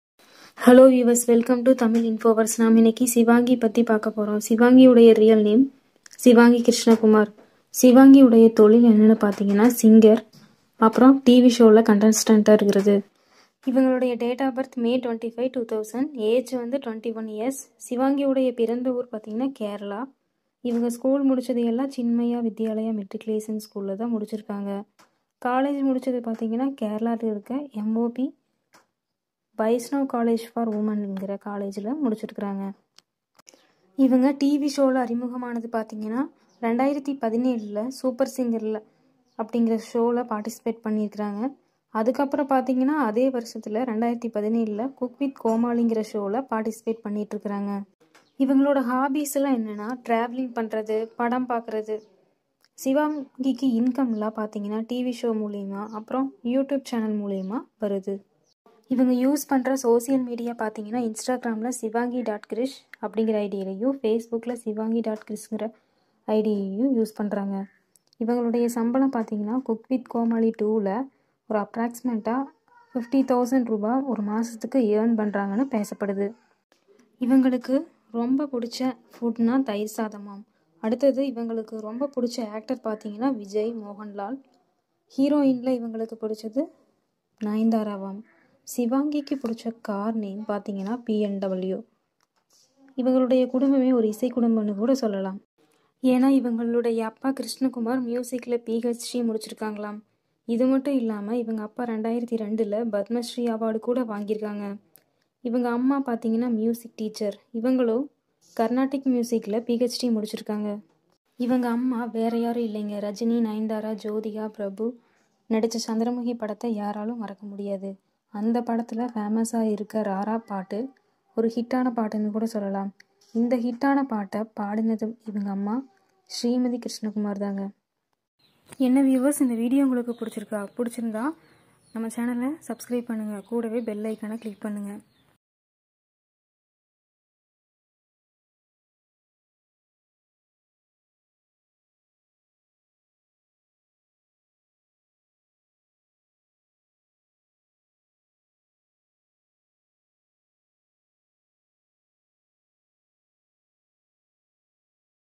Sivaangi Biography and Life Styles | Cooku with Comali Sivaangi Krishnakumar |Super Singer Shivaangi
Cooku with comali and super singer famous sivaangi krishnakumar biography ,lifestyles,favorite car unseen pictures. Shivaangi biodata